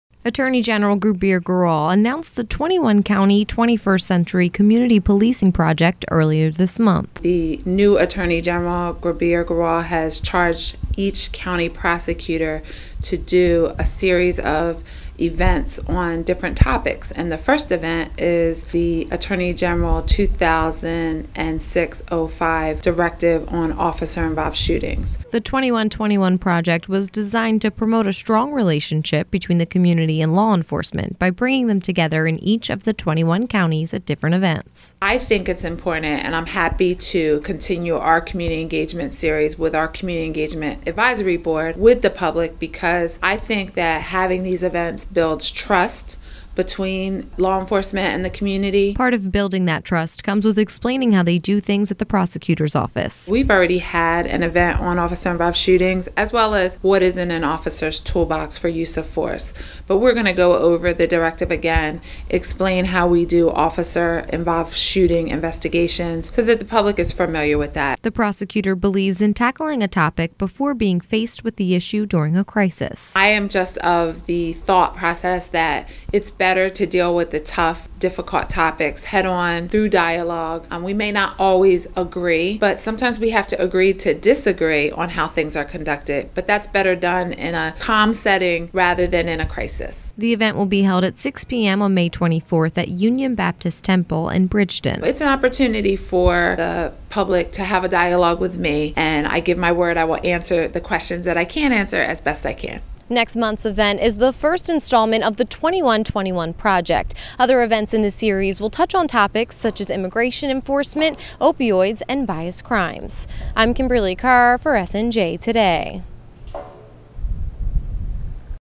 Local Prosecutor Hopes to Engage the Community | SNJ Today News
The Cumberland County Prosecutor’s Office is kicking off the “21/21 Project” with the first installment focused on police-involved shootings.